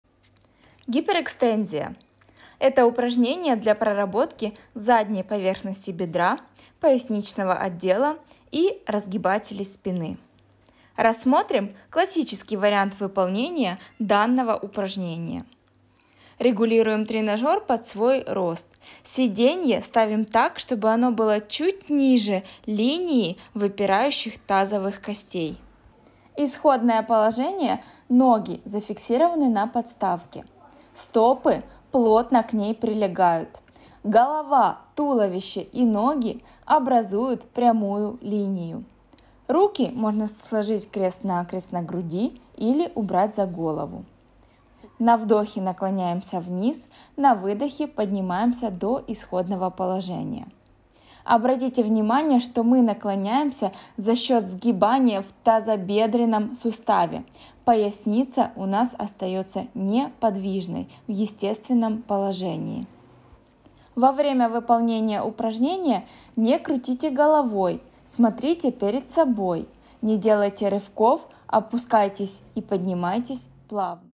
Техника упражнений: гиперэкстензия
Классический вариант выполнения гиперэкстензии. Как настроить тренажер экстензия? Как регулировать положение валика.